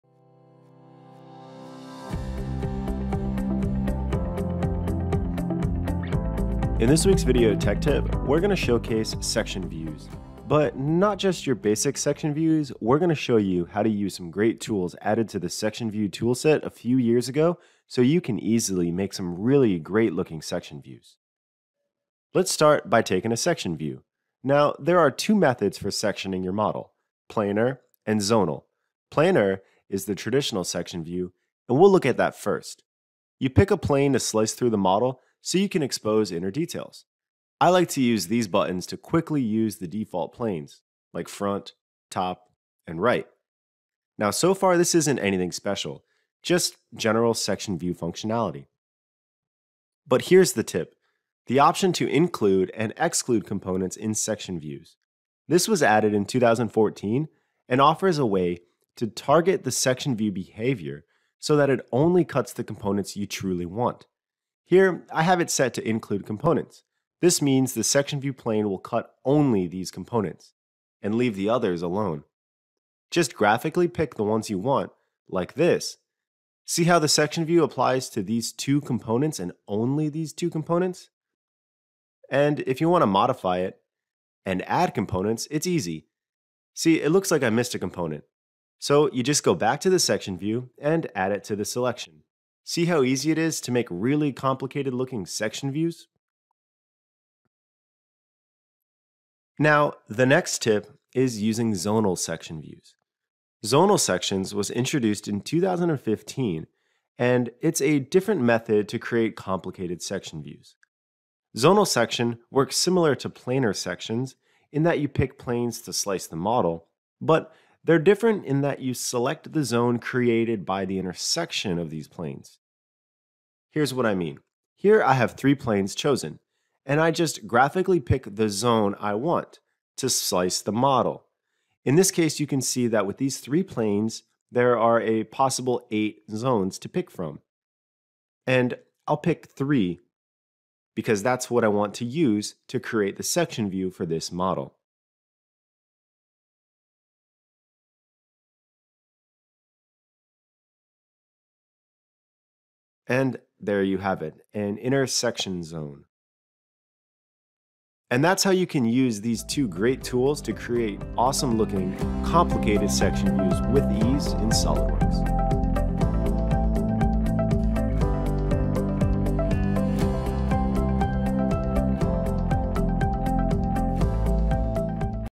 SOLIDWORKS Tech Tip: Improve Section Views
Join us as TriMech Application Engineer, Stephen Petrock, shows you how to use Section View options that were added a few years ago. After watching this you'll be able to work faster and more efficiently.